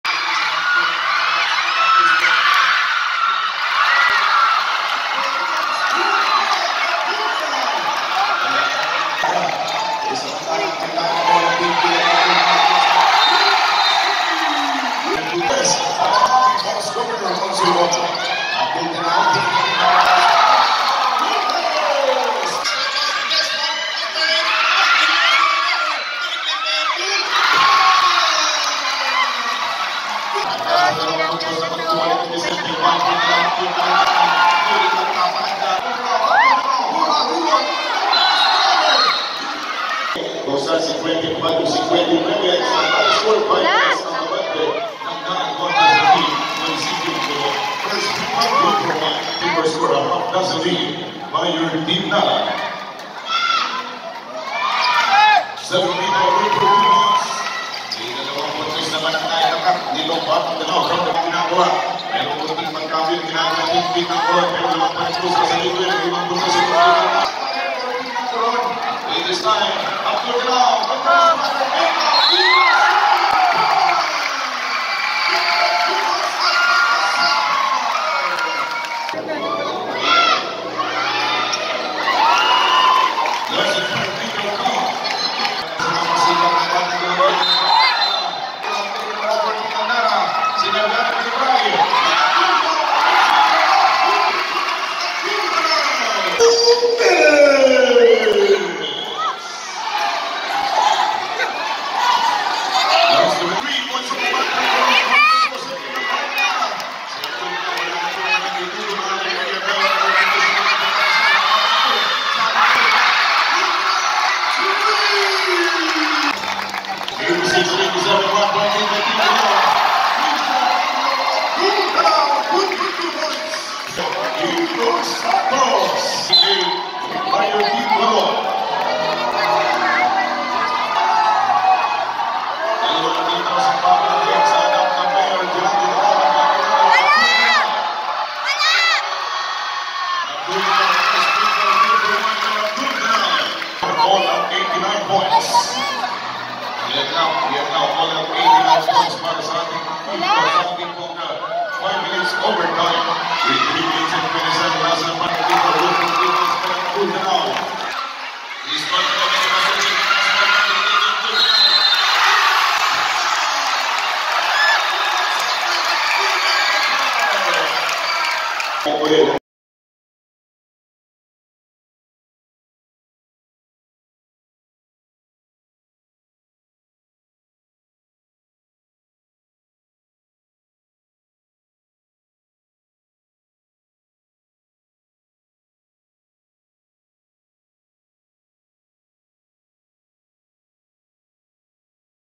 CHRISTIAN DANAO CHAMPIONSHIP HIGHLIGHTS | BARAGATAN 2023 INTER-MUNICIPALITY BASKETBALL TOURNAMENT
CHRISTIAN DANAO GAME HIGHLIGHTS VS CORON THE DEPENDING CHAMPION | CHAMPIONSHIP GAME | BARAGATAN 2023 INTER-MUNICIPALITY BASKETBALL TOURNAMENTAT PALAWAN STATE UNIVIRSITY GYMNASIUM | JUNE 20,2023
Join me in promoting and showcasing the talents of local basketball teams and players. Together, let's discover the stars of tomorrow!